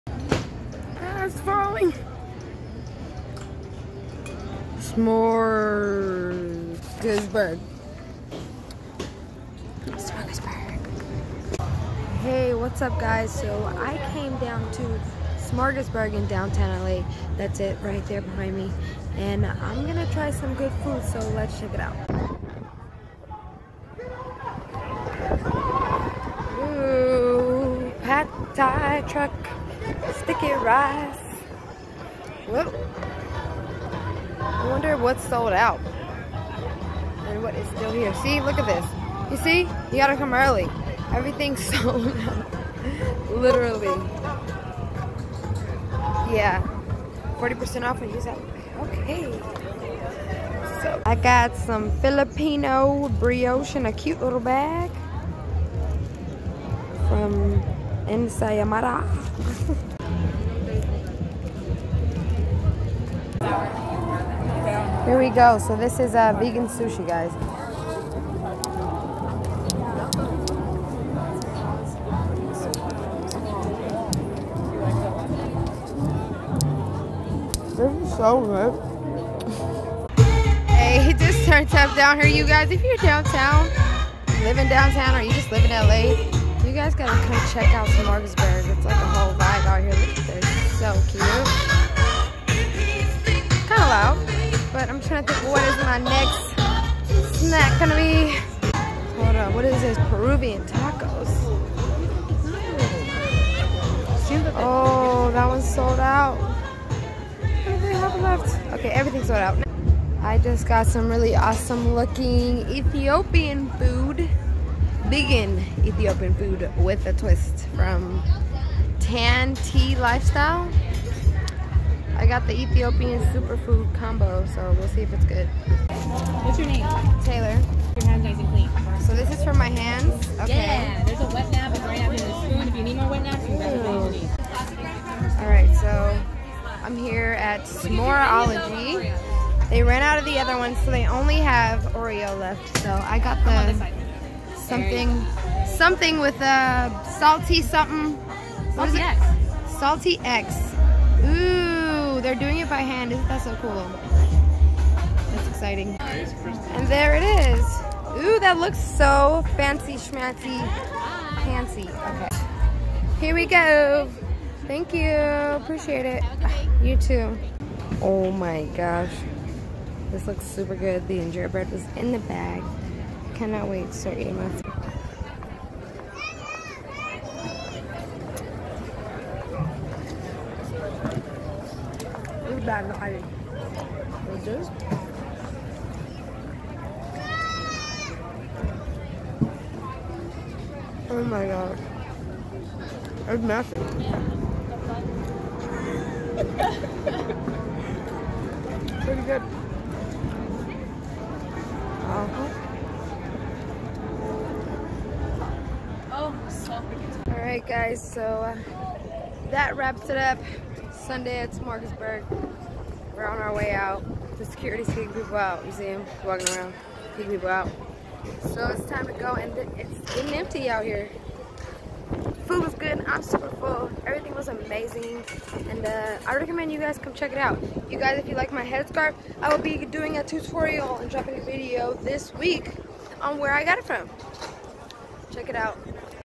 EVERYTHING SOLD OUT at Smorgasburg LA / Downtown
Gotta get to smorgasburg early on Sunday's to try to best stuff! I had a blast trying all these great foods !